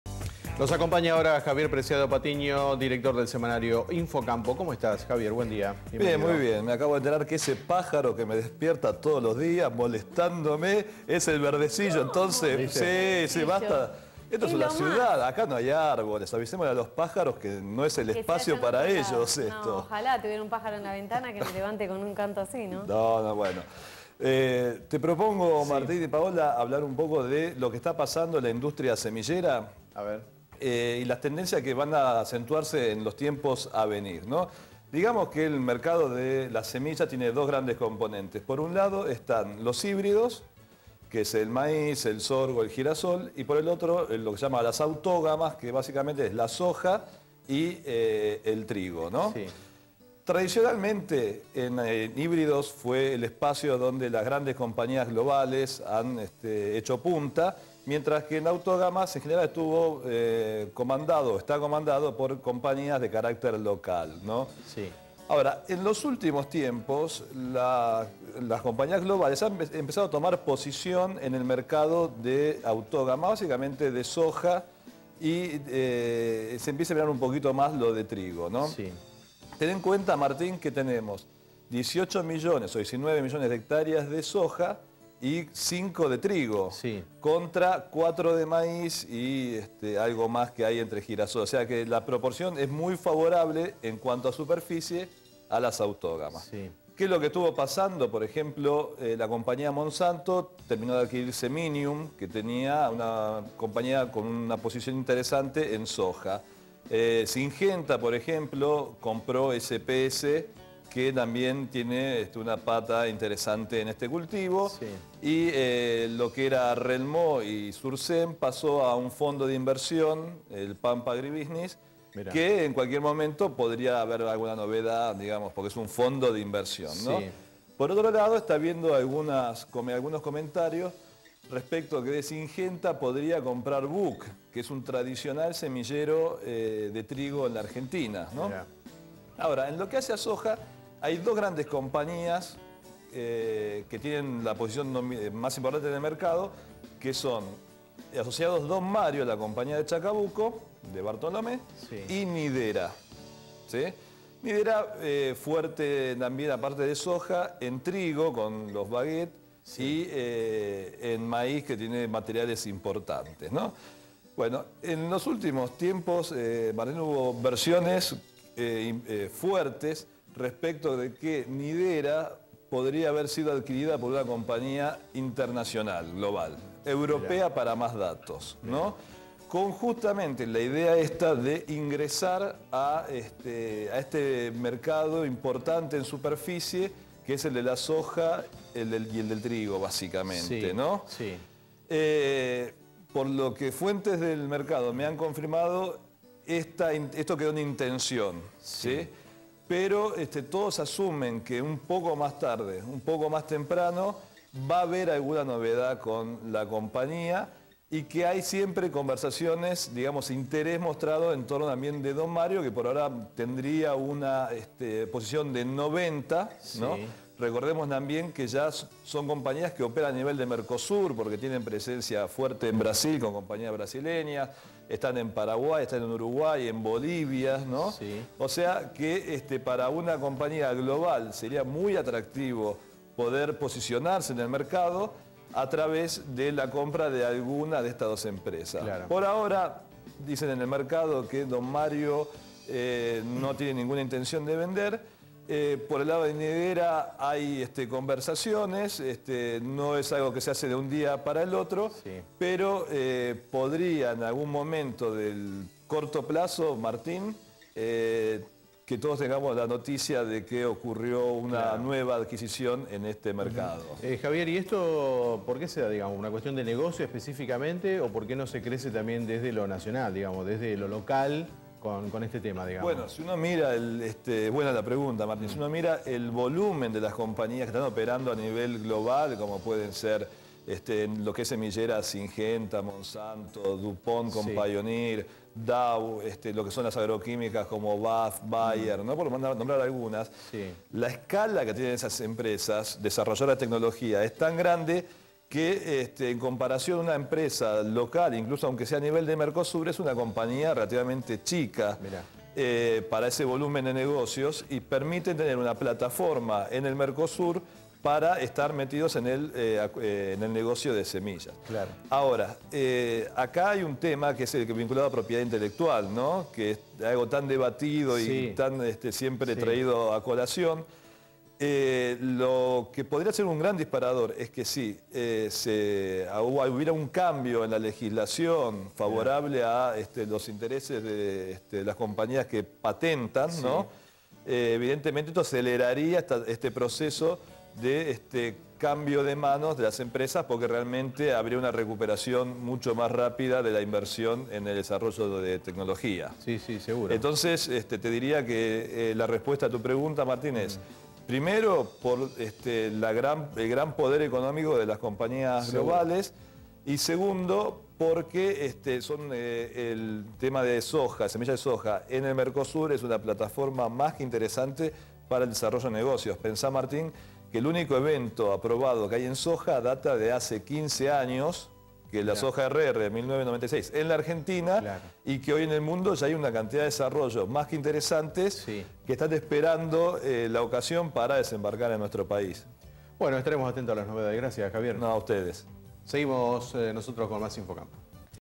Política y Agronegocios
Cambios en la industria semillera: más compras y fusiones. El desarrollo de este tema en la columna habitual de los días lunes de Javier Preciado Patiño, director periodístico del semanario Infocampo.